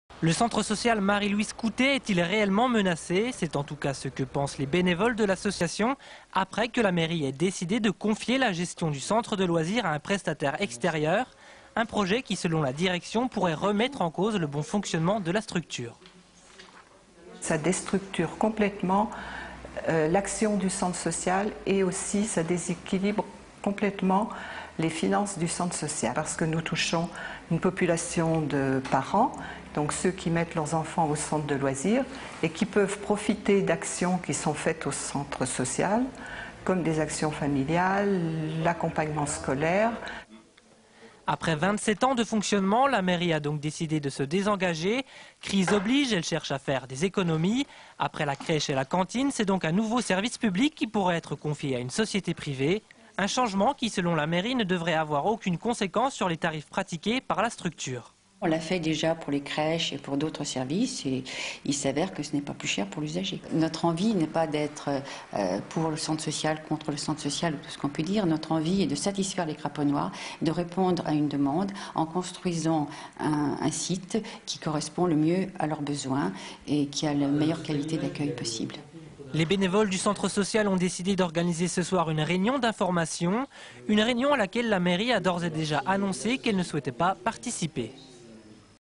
Centre Marie-Louise Couttet : La menace plane
Centre Marie-Louise Couttet : La menace plane - Lyon - Situation inconfortable pour le Centre social et culturel de Craponne. D'un côté le maire a l'intention de confier la gestion du pôle loisir à un prestataire de services et de l'autre l'agrément de la caisse d'allocation familiale risque de ne pas être assurée cette année. (Pour obtenir les droits d’exploitation commerciale de cette vidéo, veuillez contacter )